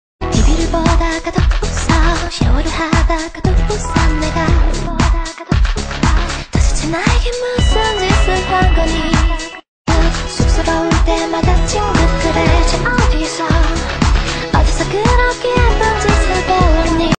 [Fancam] SNSD Yuri look at camera~ @ 100424 Super Concert
by kwonyuri125